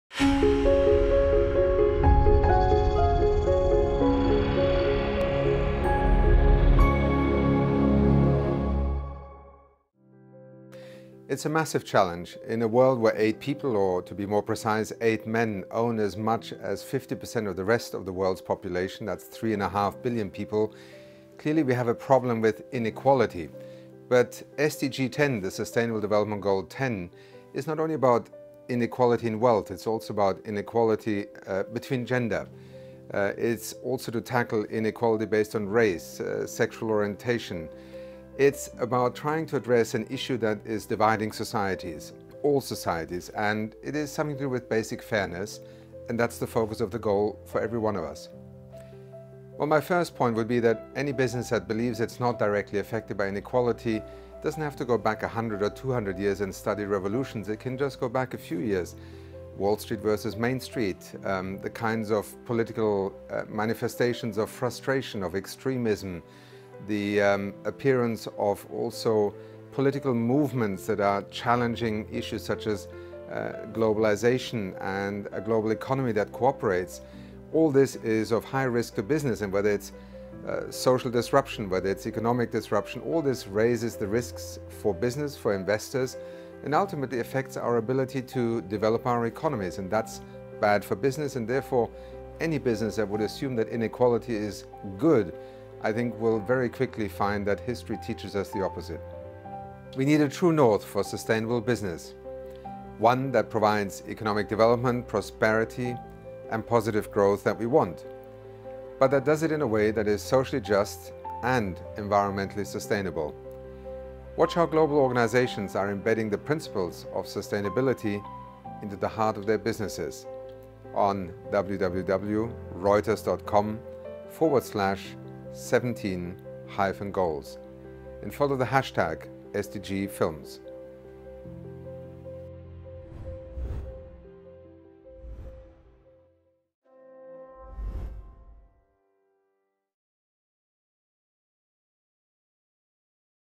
Achim Steiner on Reduced Inequalities
The Sustainable Development Goals are the blueprint to achieve a better and more sustainable future for all. In our video, UNDP Administrator Achim Steiner speaks about the importance of SDG 10, Reduced Inequalities.